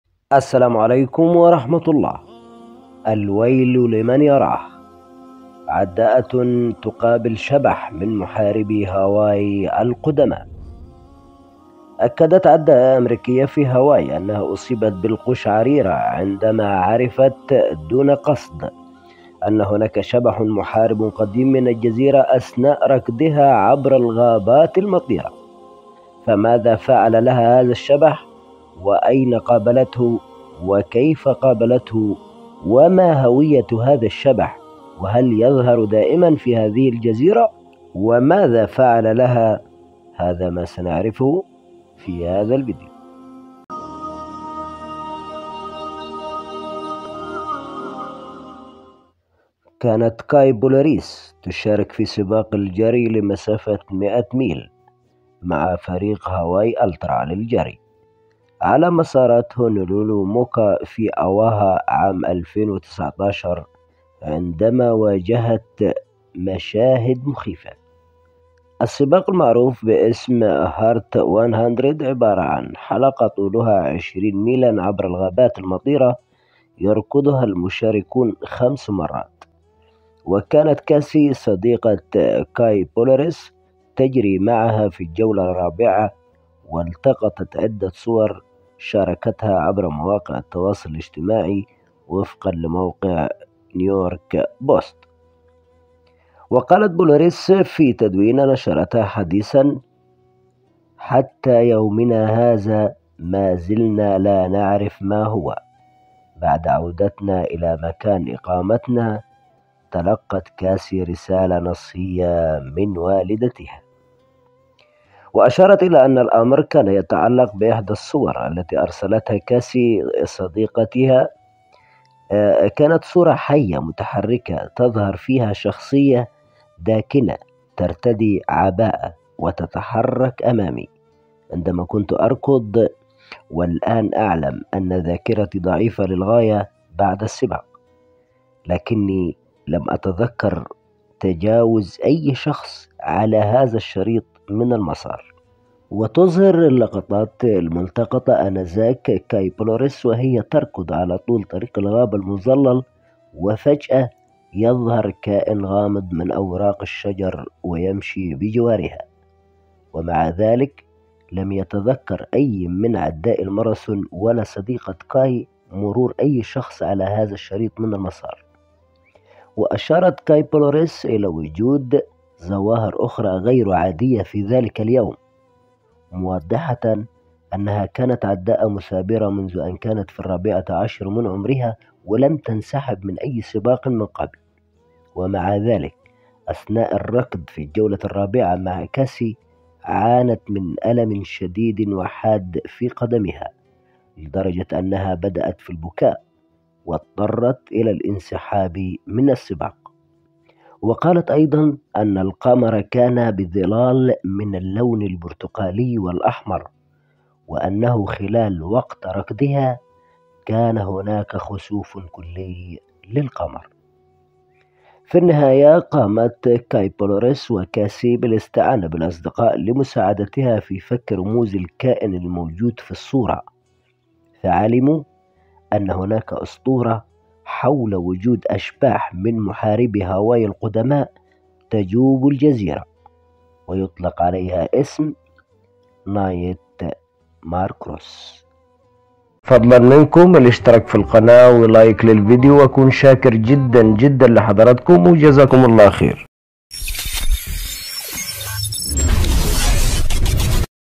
الويل لمن يراه/عداءة إمريكية تتعرض للقطات مرعبة
عداءة إمريكية تتعرض لمواجهه شبح المحاربين القدماء في جزيرة هونولولو
قناة ماذا لو قولت